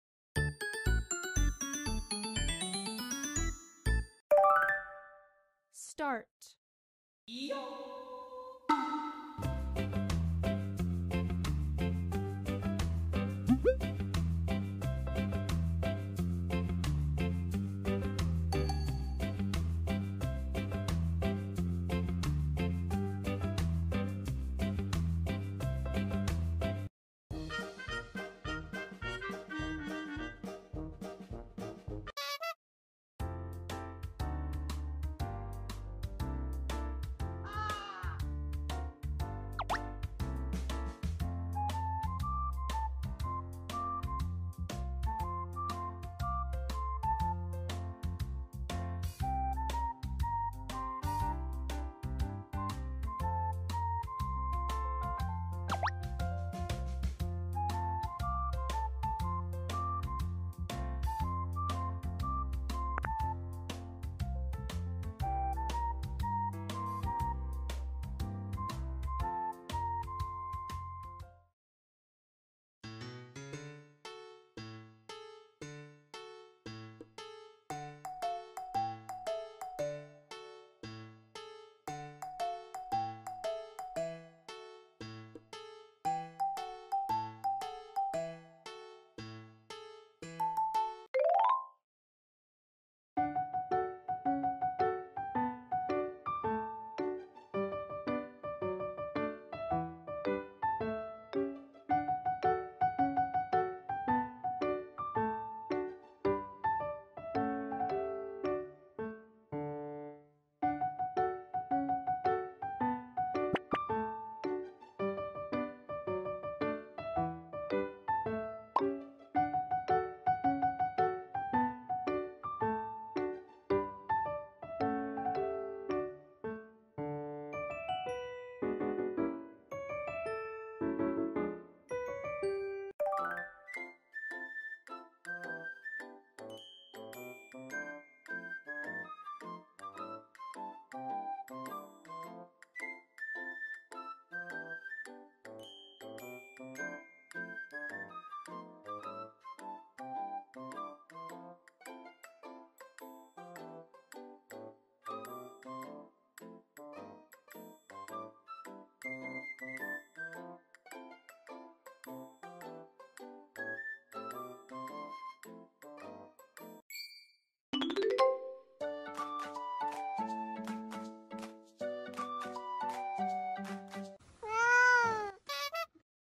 【わお！こんな可愛い猫の寝相珍しい〜🤭😻🐈‍⬛🐈】保護猫ピノちゃまとクセつよめパルムちゃま❤️
やっぱり猫たちのゴロンちゃんには、癒されます🥰🥰🥰リラックスピノパル兄弟ニャン😻😻😻💖💖💖いつも本当にありがとニャン❤️❤️❤️